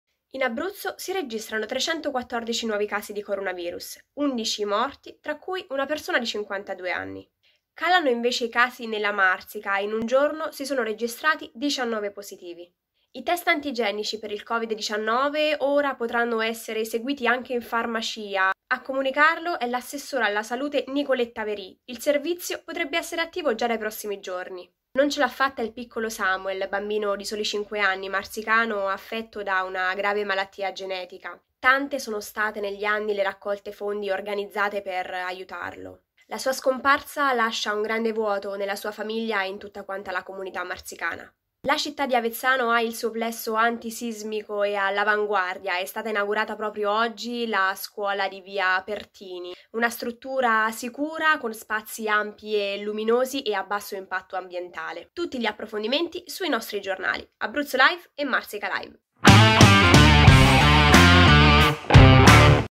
Flash Live - Le Notizie del 31 Marzo in un Minuto
Un giorno in un minuto: tutte le notizie più importanti del 31 Marzo in 60 secondi.